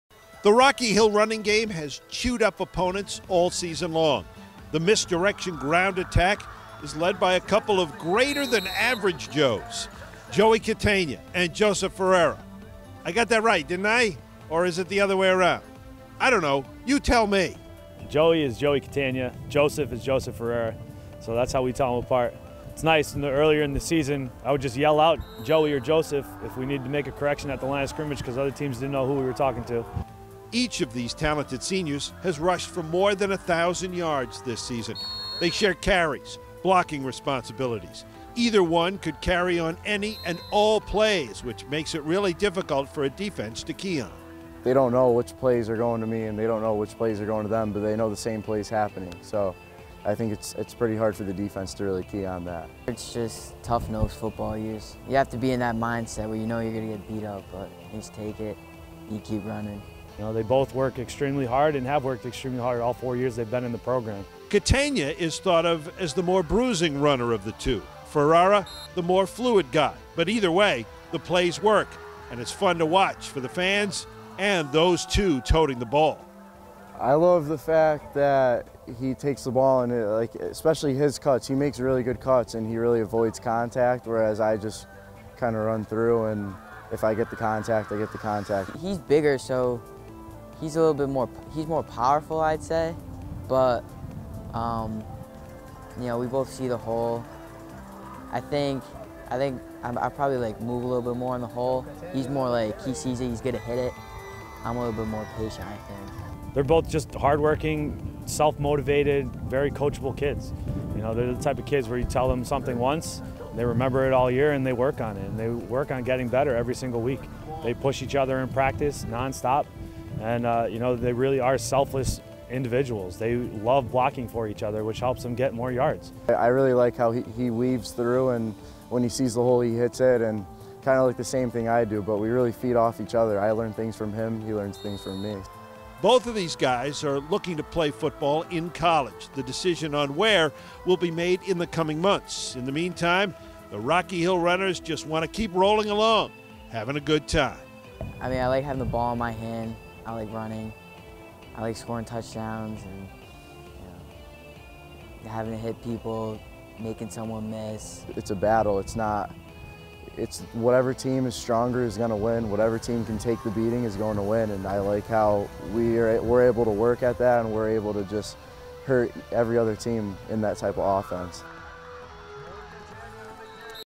Rocky Hill Football - CIAC State Semifinals Prep
ROCKY HILL TERRIERS running into the CIAC State Semifinals again St. Joes behind two unordinary Joes.